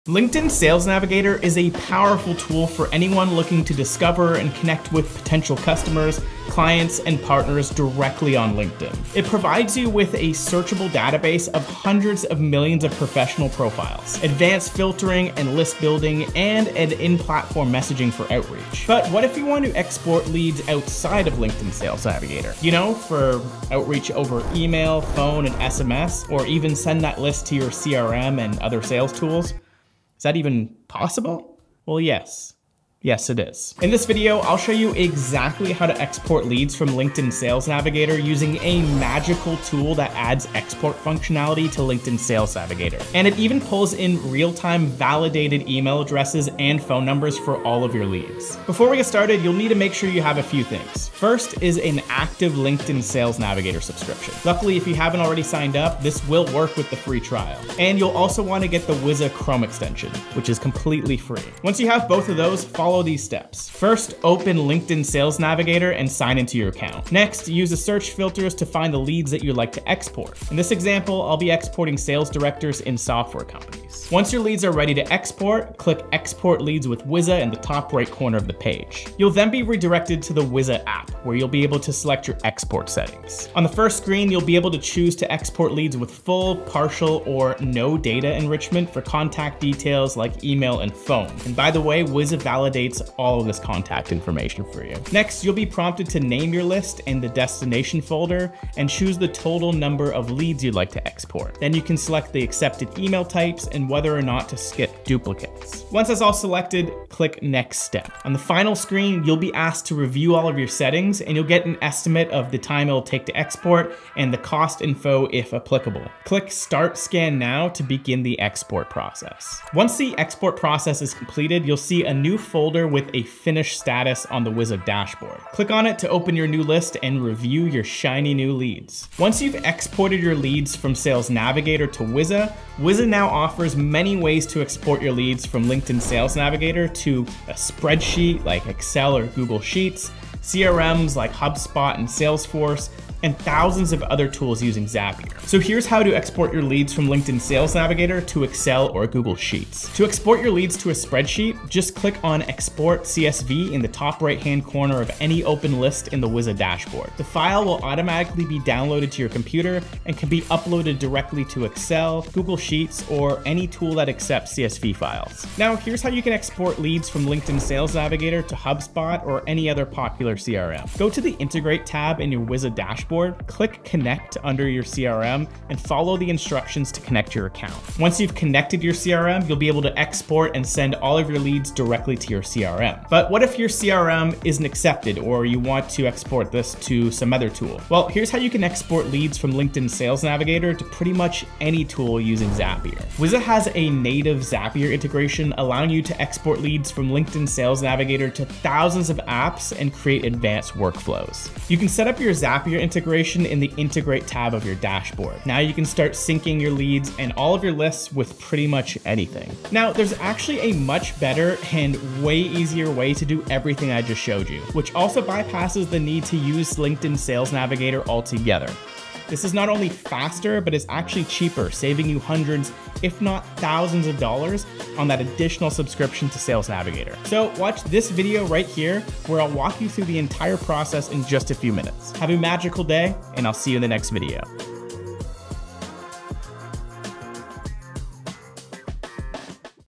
How to Export Leads From LinkedIn Sales Navigator
Export your LinkedIn Sales Navigator leads directly to a spreadsheet, CRM, or Zapier to connect with thousands of other tools — all for FREE with Wiza!

Streamline your workflow and save time with just a few clicks

Get the free Wiza chrome extension for LinkedIn and Sales Navigator

What we cover:
0:00 - Intro
0:48 - Set up Sales Navigator + Wiza
1:14 - Export Sales Navigator leads with Wiza
2:25 - Export leads to a spreadsheet
2:45 - Export leads to your CRM
3:08 - Connect Sales Navigator to Zapier